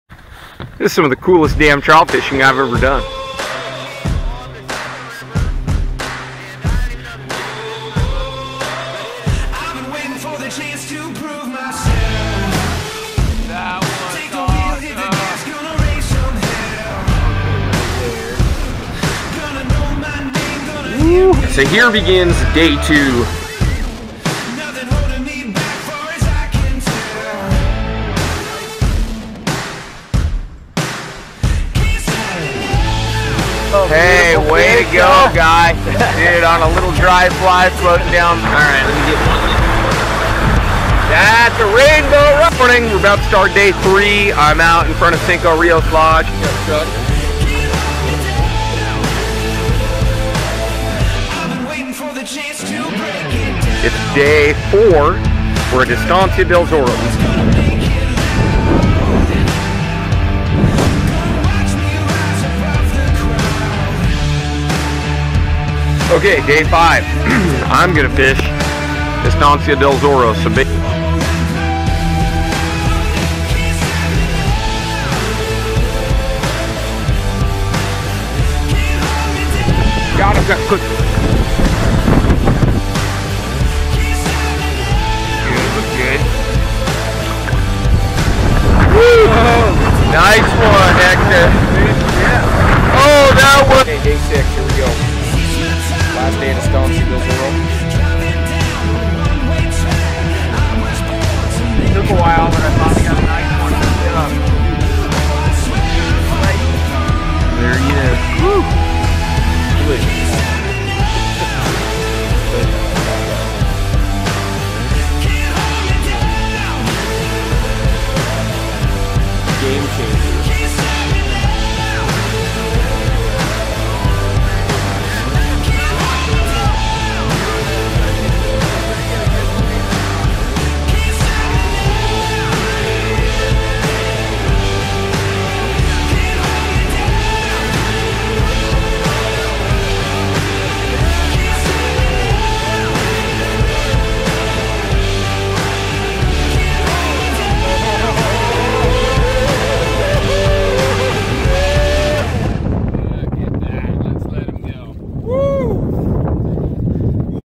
Patagonia Trout Trailer - Day by Day Video Series
This is the trailer for our Day by Day video series where we will Vlog each and every day. Fish or no fish Red's Fly Shop will bring it to ya straight.